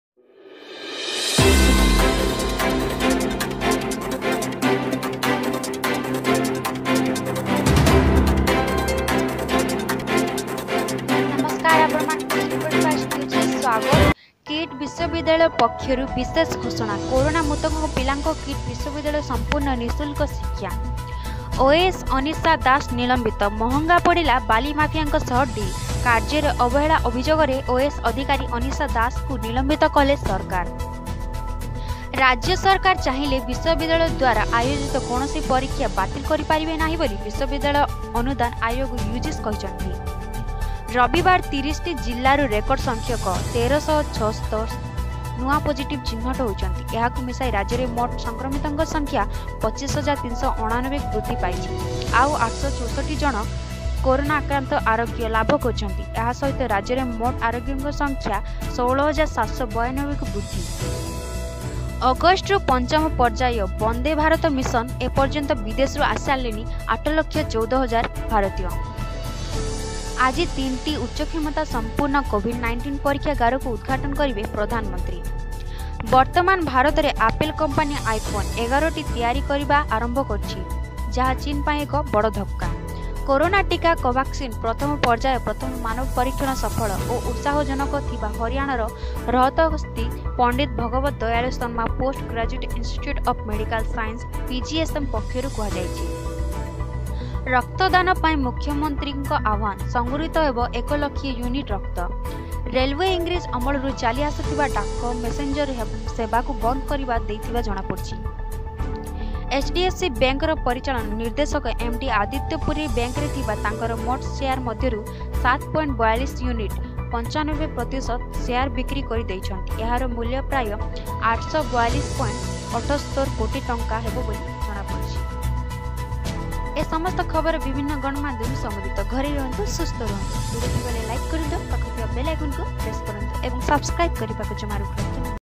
ଆଜିର ସମାଚାର Dt-27/07/2020 // Report by- Amiya kumar sahu
*ଖବର ଆଜିର*
ତାରିଖ ୨୭/୦୭/୨୦,ରବିବାର

୧-କିଟ୍‌ ବିଶ୍ୱବିଦ୍ୟାଳୟ ପକ୍ଷରୁ ବିଶେଷ ଘୋଷଣା; କରୋନା ମୃତକଙ୍କ ପିଲାଙ୍କୁ କିଟ୍‌ ବିଶ୍ୱବିଦ୍ୟାଳୟରେ ସଂପୂର୍ଣ୍ଣ ନିଃଶୁଳ୍କ ଶିକ୍ଷା।

୨-ଓଏଏସ୍ ଅନୀଷା ଦାସ ନିଲମ୍ବିତ । ମହଙ୍ଗା ପଡିଲା ବାଲି ମାଫିଆଙ୍କ ସହ ଡ଼ିଲ । କାର୍ଯ୍ୟରେ ଅବହେଳା ଅଭିଯୋଗରେ ଓଏଏସ୍ ଅଧିକାରୀ ଅନୀଷା ଦାସଙ୍କୁ ନିଲମ୍ବିତ କଲେ ସରକାର ।

୩-ରାଜ୍ୟ ସରକାର ଚାହିଁଲେ ବିଶ୍ୱବିଦ୍ୟାଳୟ ଦ୍ୱାରା ଆୟୋଜିତ କୌଣସି ପରୀକ୍ଷା ବାତିଲ କରିପାରିବେ ନାହିଁ ବୋଲି ବିଶ୍ୱବିଦ୍ୟାଳୟ ଅନୁଦାନ ଆୟୋଗ, ୟୁଜିସି କହିଛି।

୪-ରବିବାର ୩୦ ଜିଲାରୁ ରେକର୍ଡ ସଂଖ୍ୟକ ୧୩୭୬ ନୂଆ ପଜିଟିଭ୍‌ ଚିହ୍ନଟ ହୋଇଛନ୍ତି। ଏହାକୁ ମିଶାଇ ରାଜ୍ୟରେ ମୋଟ ସଂକ୍ରମିତଙ୍କ ସଂଖ୍ୟା ୨୫,୩୮୯କୁ ବୃଦ୍ଧି ପାଇଛି।
ଆଉ ୮୬୪  ଜଣ କରୋନା ଆକ୍ରାନ୍ତ ଆରୋଗ୍ୟ ଲାଭ କରିଛନ୍ତି। ଏହାସହିତ ରାଜ୍ୟରେ ମୋଟ ଆରୋଗ୍ୟଙ୍କ ସଂଖ୍ୟା ୧୬୭୯୨କୁ ବୃଦ୍ଧି

୫-୧ ଅଗଷ୍ଟରୁ ପଞ୍ଚମ ପର୍ଯ୍ୟାୟ ‘ବନ୍ଦେ ଭାରତ’ ମିଶନ, ଏପର୍ଯ୍ୟନ୍ତ ବିଦେଶରୁ ଆସିସାରିଲେଣି ୮ଲକ୍ଷ ୧୪ ହଜାର ଭାରତୀୟ।

୬-ଆଜି ୩ଟି ଉଚ୍ଚକ୍ଷମତା ସଂପନ୍ନ କୋଭିଡ-୧୯ ପରୀକ୍ଷାଗାରକୁ ଉଦ୍‌ଘାଟନ କରିବେ ପ୍ରଧାନମନ୍ତ୍ରୀ।

୭-ବର୍ତ୍ତମାନ ଭାରତରେ ଆପଲ୍ କମ୍ପାନୀ ଆଇଫୋନ୍-୧୧ ତିଆରି କରିବା ଆରମ୍ଭ କରିଛି, ଯାହା ଚୀନ୍ ପାଇଁ ଏକ ବଡ଼ ଧକ୍କା ।

୮-କରୋନା ଟିକା କୋଭାକ୍ସିନର ପ୍ରଥମ ପର୍ଯ୍ୟାୟ ପ୍ରଥମ ମାନବ ପରୀକ୍ଷଣ ସଫଳ ଓ ଉତ୍ସାହଜନକ ଥିବା ହରିୟାଣାର ରୋହତକସ୍ଥିତ ପଣ୍ଡିତ ଭଗବତ ଦୟାଲ ଶର୍ମା ପୋଷ୍ଟ ଗ୍ରାଜୁଏଟ ଇନଷ୍ଟିଚ୍ୟୁଟ ଅଫ ମେଡିକାଲ ସାଇନ୍ସ (ପିଜିଆଇଏମ୍‍ଏସ୍‍) ପକ୍ଷରୁ କୁହାଯାଇଛି।

୯-ରକ୍ତଦାନ ପାଇଁ ମୁଖ୍ୟମନ୍ତ୍ରୀଙ୍କ ଆହ୍ୱାନ:ସଂଗୃହୀତ ହେବ ୧ ଲକ୍ଷ ୟୁନିଟ୍‌ ରକ୍ତ।

୧୦-ରେଲଓ୍ବେ ଇଂରେଜ ଅମଳରୁ ଚାଲି ଆସୁଥିବା ଡାକ ମେସେଞ୍ଜର ସେବାକୁ ବନ୍ଦ କରି ଦେଇଥିବା ଜଣାପଡିଛି।

୧୧-ଏଚ୍‌ଡିଏଫ୍‌ସି ବ୍ୟାଙ୍କର ପରିଚାଳନା ନିର୍ଦ୍ଦେଶକ(ଏମ୍‌ଡି) ଆଦିତ୍ୟ ପୁରୀ ବ୍ୟାଙ୍କରେ ଥିବା ତାଙ୍କର ମୋଟ ଶେୟାର ମଧ୍ୟରୁ ୭.୪୨ ନିୟୁତ(୯୫%) ଶେୟାର ବିକ୍ରି କରି ଦେଇଛନ୍ତି। ଏହାର ମୂଲ୍ୟ ପ୍ରାୟ ୮୪୨.୮୭ କୋଟି ଟଙ୍କା ହେବ ବୋଲି ଜଣାପଡିଛି।